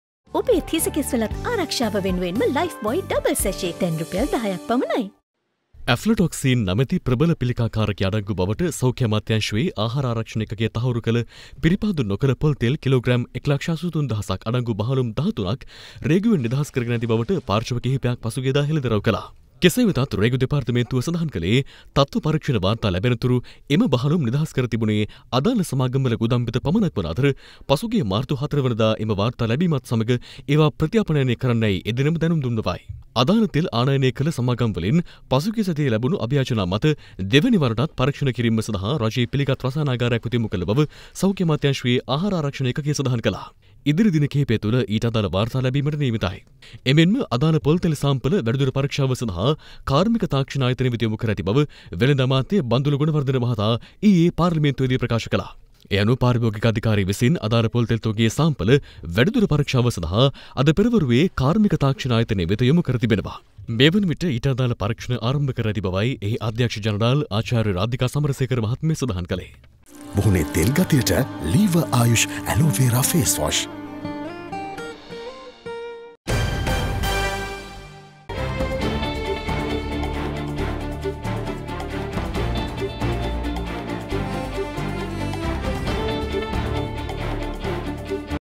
පිළිකා කාරකය අඩංගු පොල්තෙල් අද වැඩිදුර පරීක්ෂාවට...
Probes into coconut oil containing carcinogen to continue today